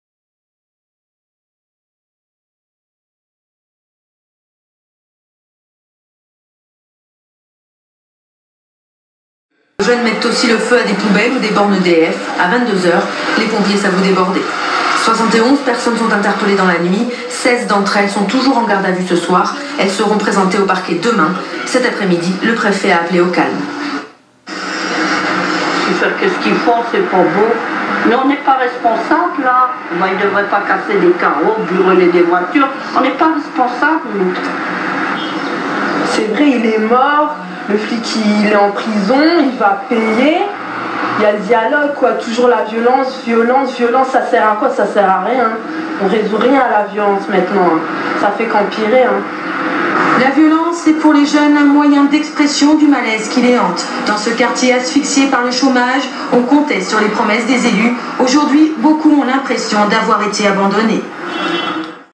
France 2/3 - 8 Avril 2000 - Archive Télé 353 du Fantôme du Temps
8 Avril 2000 .
Une première en ce service militaire en voie de disparition ( Gg en cours d'année , n'y échappera pas ) ; la journée d'appel de préparation à la défense pour les filles ... S'en suit sur France 3 ( sans doute au journal du soir ) la suite des émeutes dans la banlieue Lilloise  après la mort d'un " jeune " ( pas blanc comme neige , au sens figuré évidemment ) ; lors d'une intervention policière ...
Malheureusement , la séquence de la journée d'appel à la défense a été coupée par YouTube une fois encore au nom du sacro-saint droit d'auteur ...